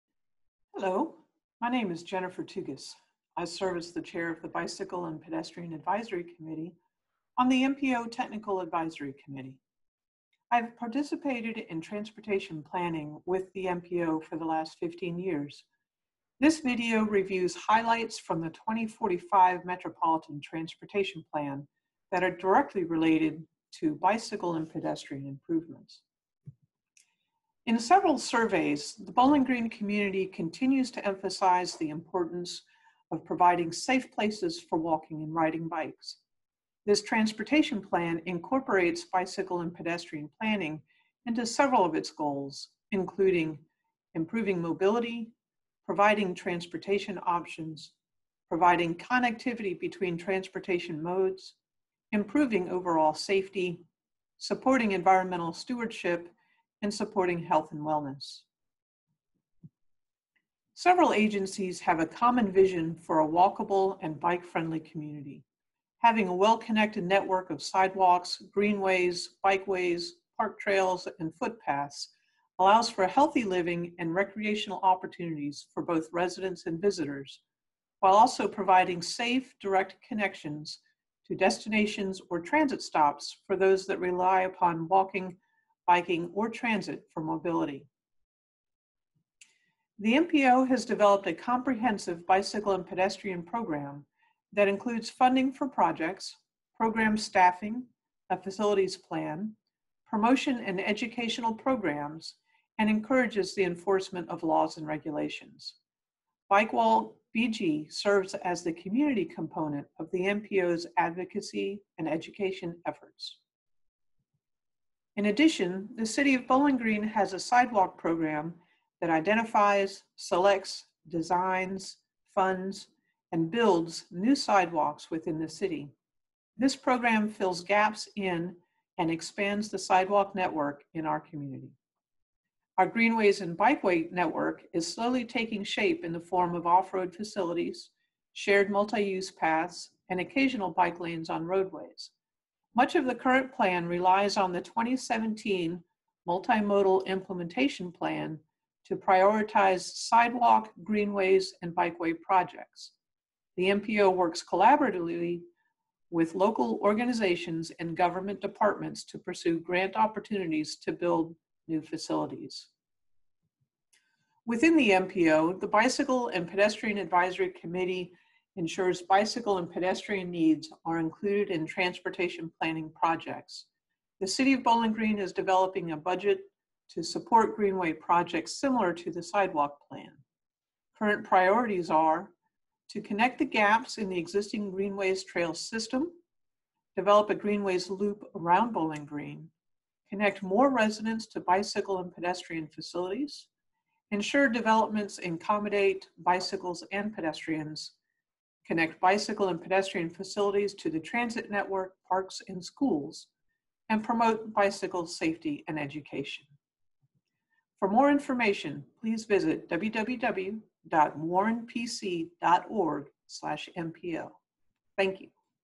2045 MTP | Bicycle & Pedestrian Priorities
Learn more about the future priorities for improving biking and walking in Bowling Green and Warren County.